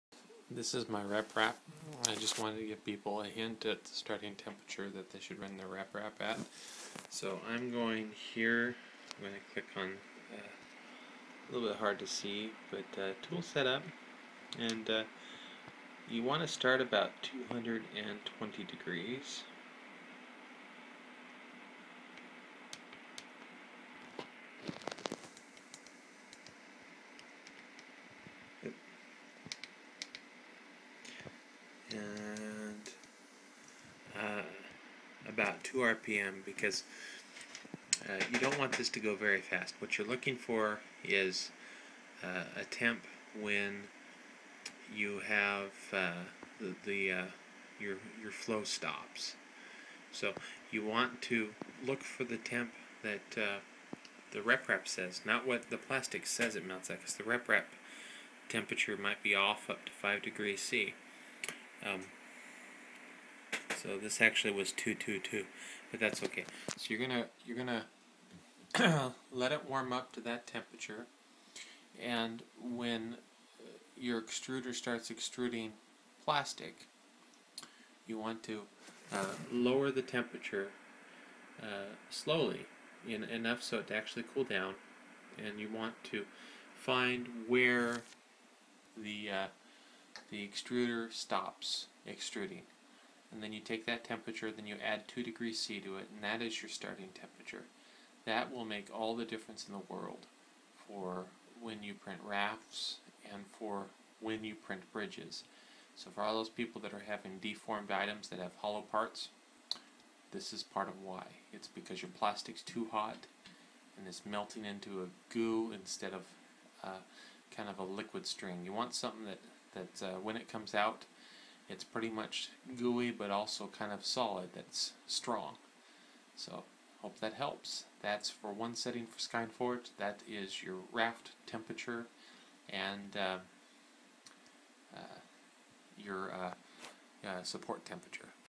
finding reprap temps to use for skeinforge setting raft temp
These are instructions for people having trouble setting raft temp for there reprap. raise temp on extruder to  220 for BPA, other temp for other materials and slowly lower temp over a period of a few minutes to see when plastic stops flowing. take that temp add 2 degrees c to it and that is your raft and first layer temp. all other layers should be around 5 degrees c higher. this will solve poor builds until 3d simple software wizard is released, with a built in wizard to set optimum settings.